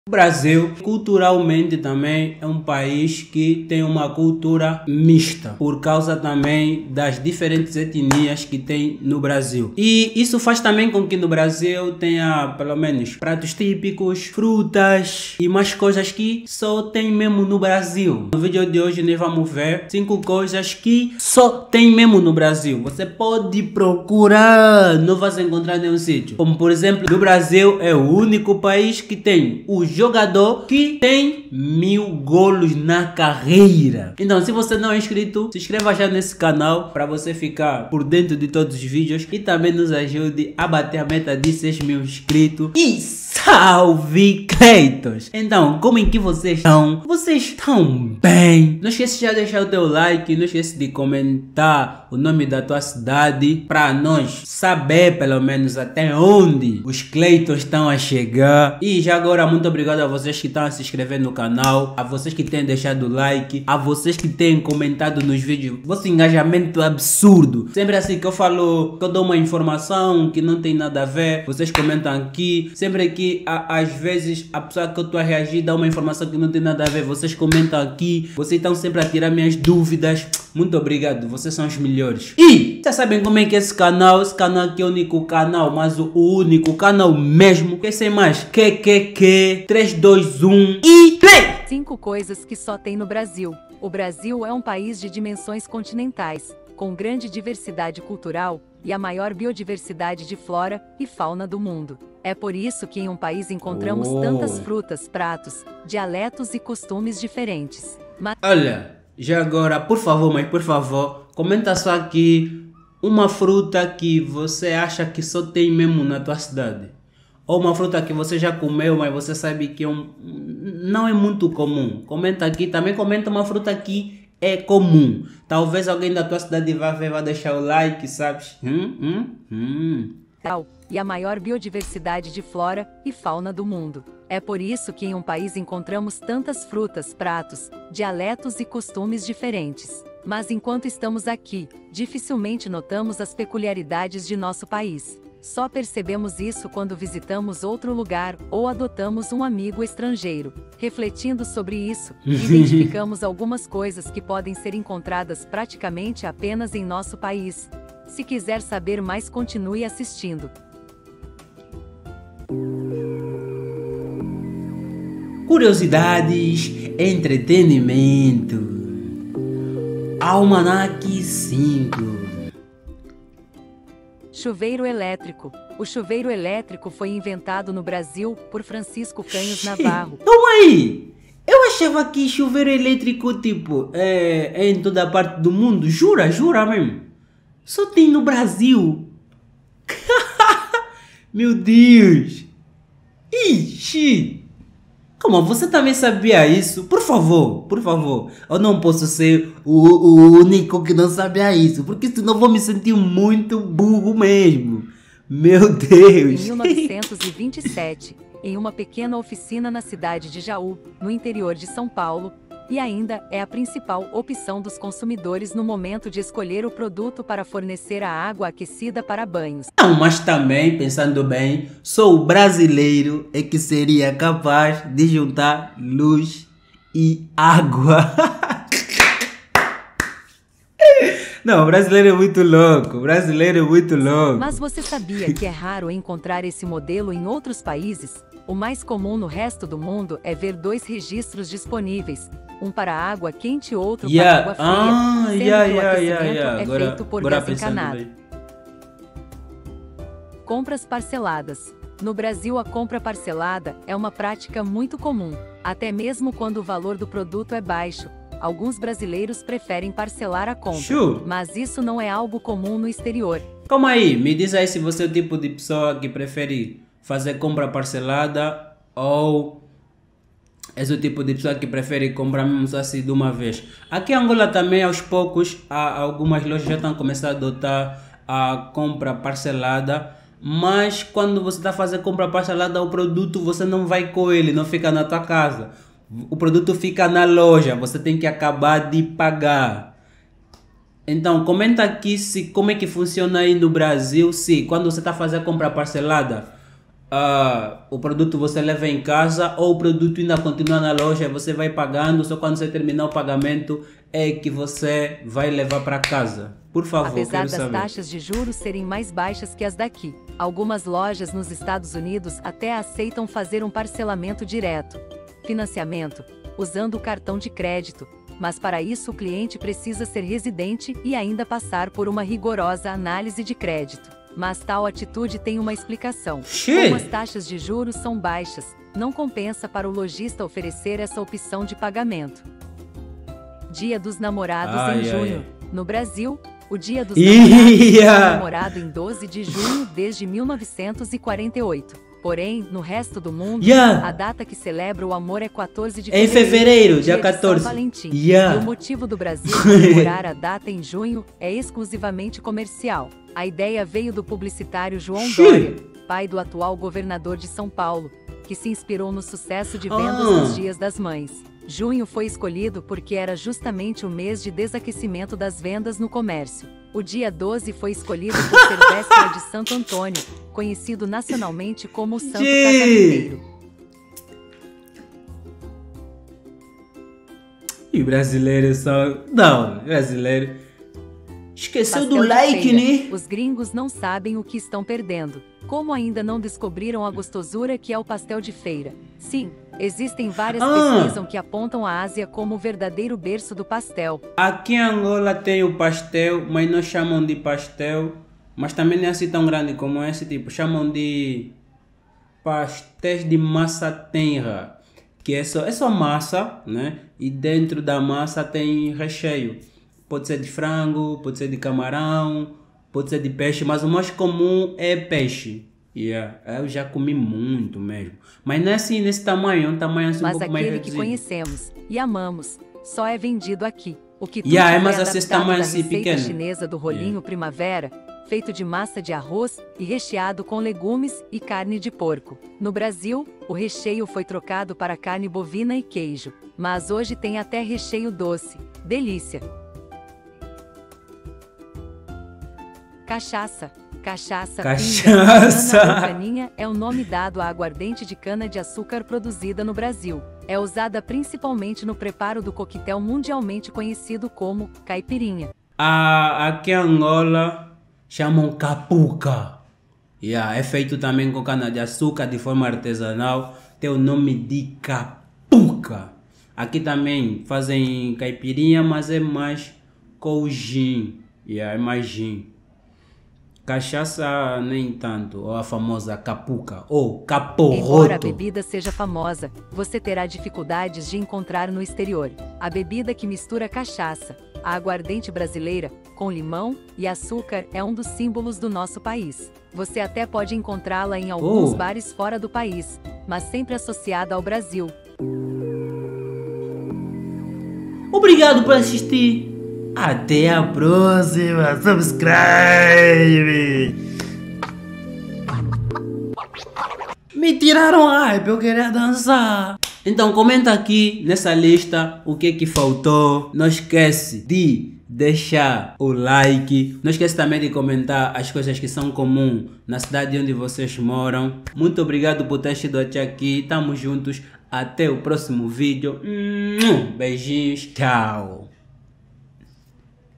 VOCÊ SABIA? O BRASIL É O ÚNICO PAÍS QUE TEM CHUVEIRO ELÉTRICO e mais umas 4 COISAS !
NÃO ACREDITO! O BRASIL É O ÚNICO PAÍS QUE TEM ESSAS COISAS?

MEU BLOG que Tá Falido😂😂😂